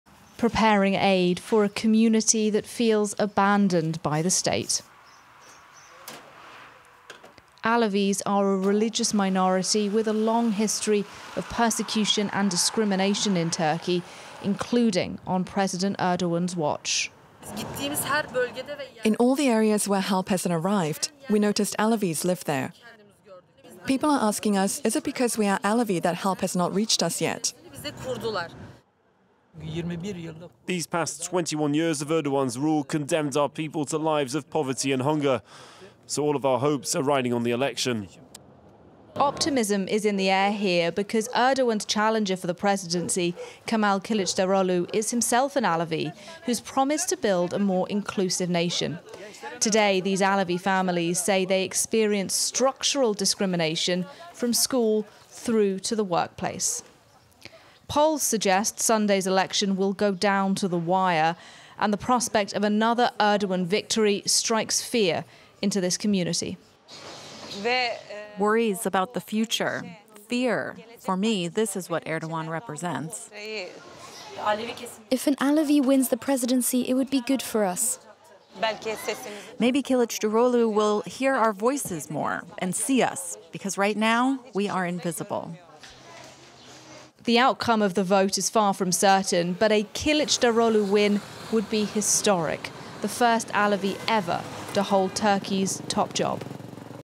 FRANCE 24 report: Alevi minority's hopes for Turkey's future ahead of elections • FRANCE 24
Days ahead of presidential and parliamentary elections, Turkey is at a crossroads. President Recep Tayyip Erdogan is seeking an unprecedented third term in power, but is facing a serious challenger: technocrat Kemal Kilicdaroglu, who is a member of the Muslim Alevi minority. His faith and identity has been a major talking point during the campaign. Ahead of the vote, FRANCE 24’s team went to meet Alevis who survived February's devastating earthquakes to listen to their story and hear their hopes for Turkey’s future.